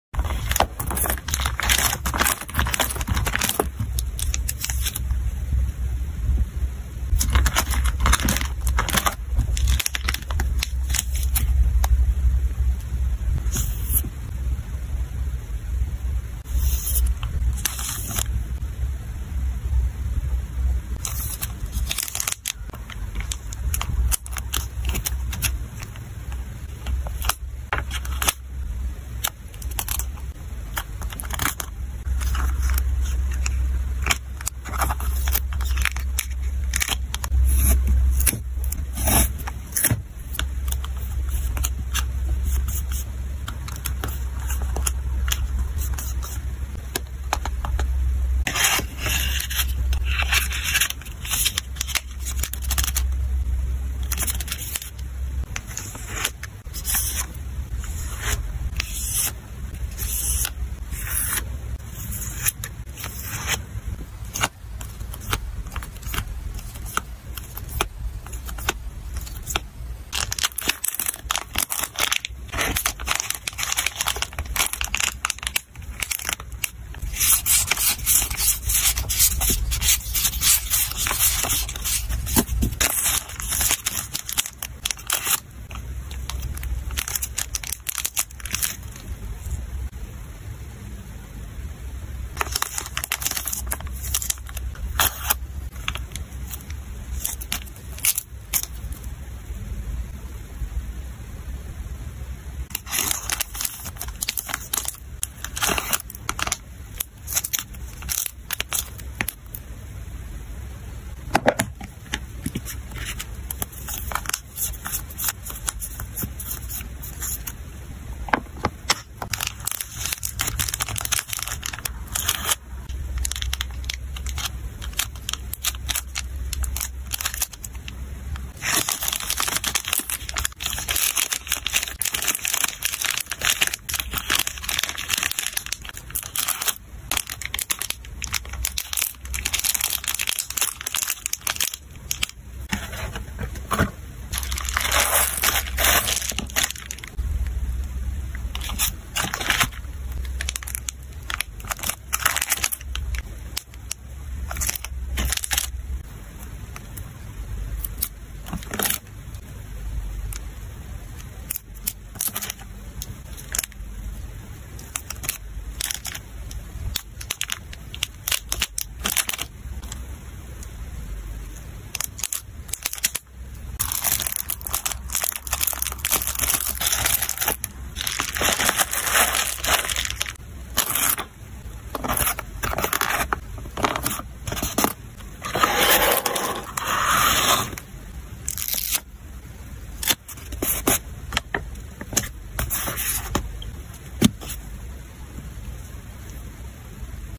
vlog丨沉浸式打包随心配丨白噪音丨助眠向丨kpop五代团小卡福袋
大家好，我是xxxifs
五代博爱
沉浸式打包小卡
本命朴成训